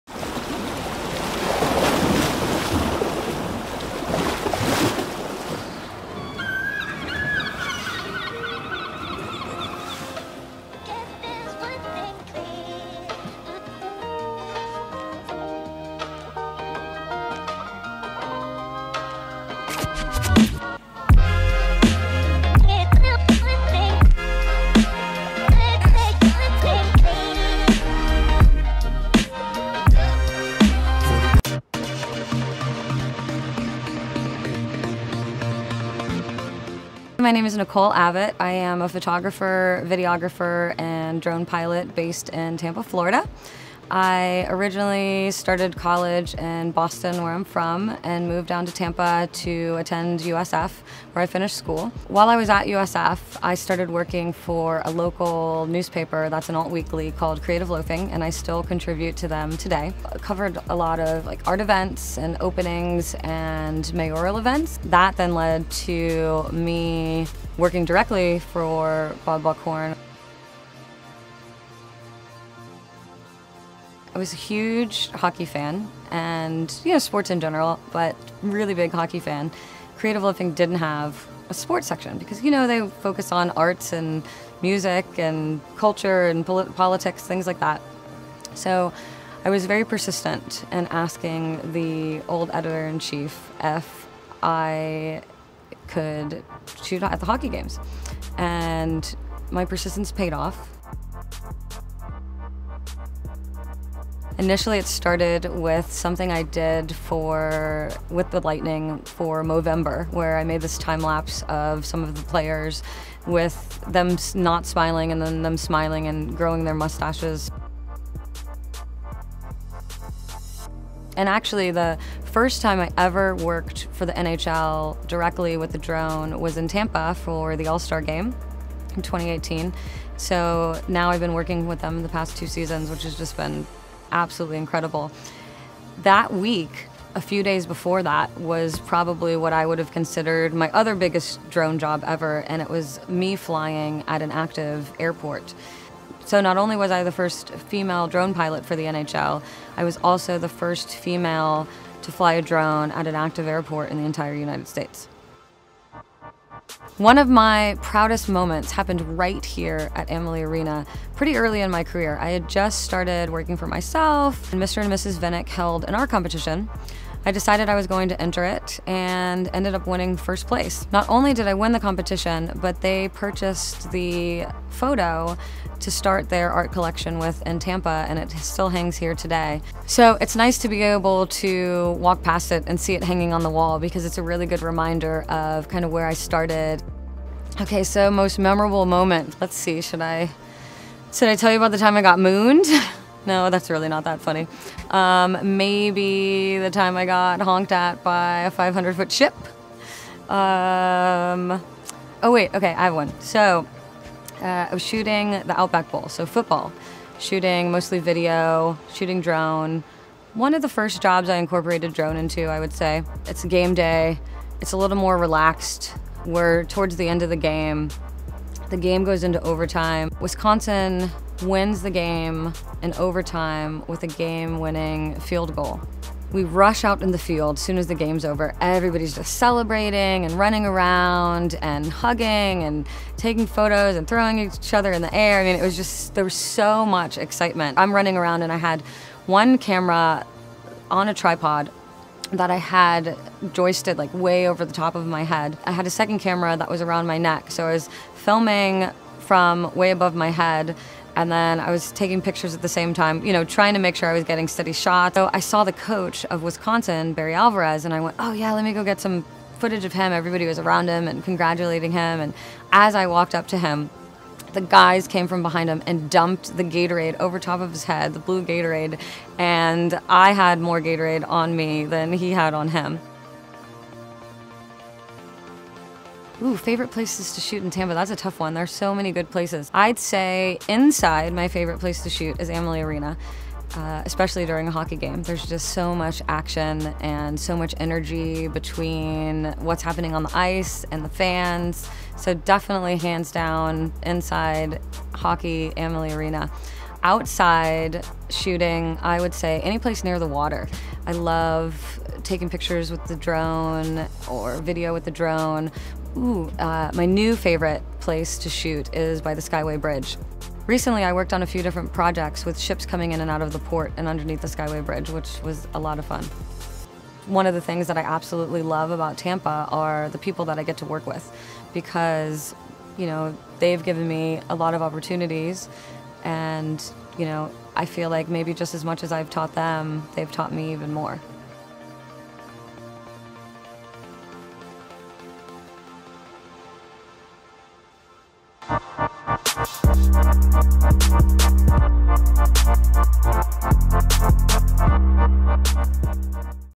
Film Tampa Bay Spotlight | Drone Pilot, Nicole Abbett
Last month, we sat down to chat with the first female drone pilot for the NHL, as well as the first female drone pilot to fly at an active airport. Nicole Abbett discusses the evolution of her career in Tampa Bay, and how drones have helped her soar to some of the most exciting jobs she's ever had. To learn more about Nicole Abbett, follow her on Instagram @nabbett, or check out fromwhereifly.com.